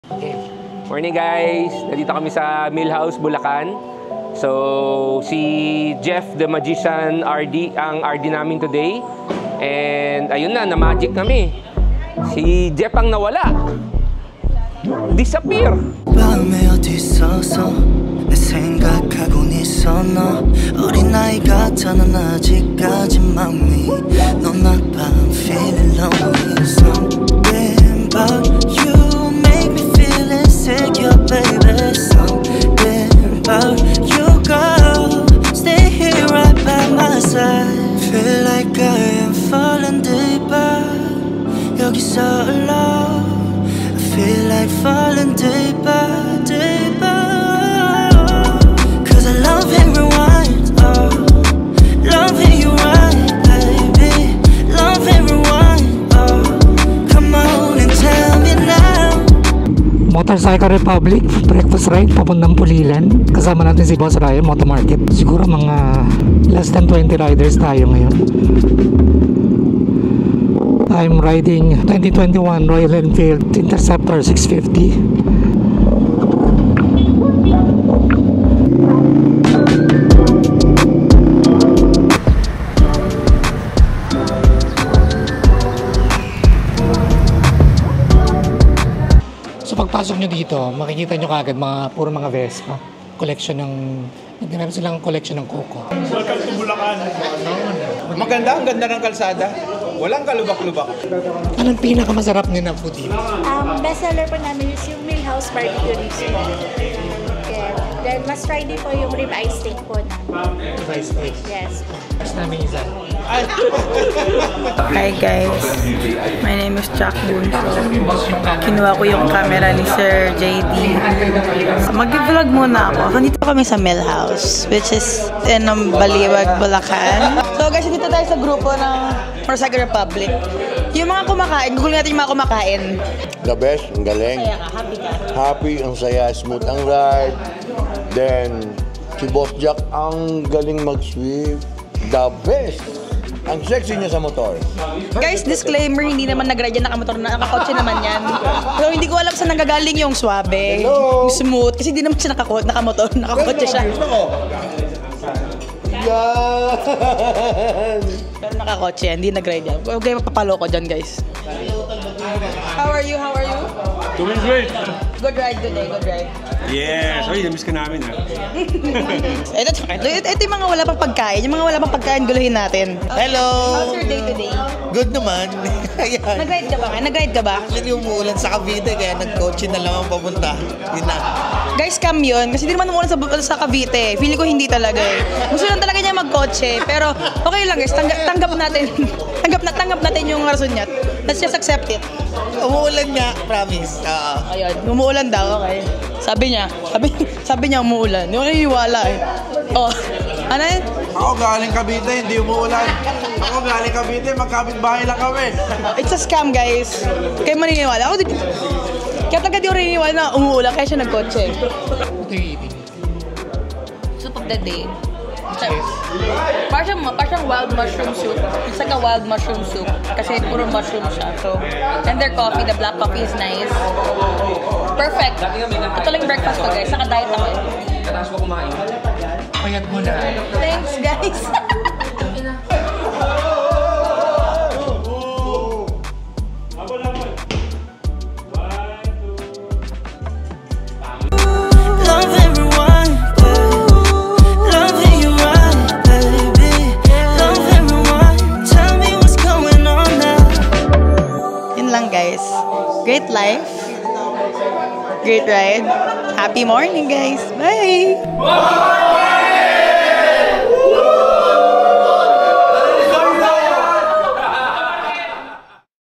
Mill House Bulacan feat Jacq Buncio
Welcome to Mill House Bulacan, a unique dining destination located in the heart of Bustos, Bulacan. Nestled in a beautifully restored heritage house, Mill House offers a perfect blend of traditional Filipino flavors and modern culinary innovations. Whether you’re in the mood for a hearty meal, a cozy brunch, or a laid-back coffee break, this restaurant provides the ideal atmosphere with its rustic charm and warm ambiance.

Enjoy their signature dishes that highlight local ingredients and recipes passed down through generations. Perfect for family gatherings, romantic dinners, or casual outings, Mill House is not just a restaurant – it’s an experience.

Come visit and savor the taste of Bulacan at Mill House!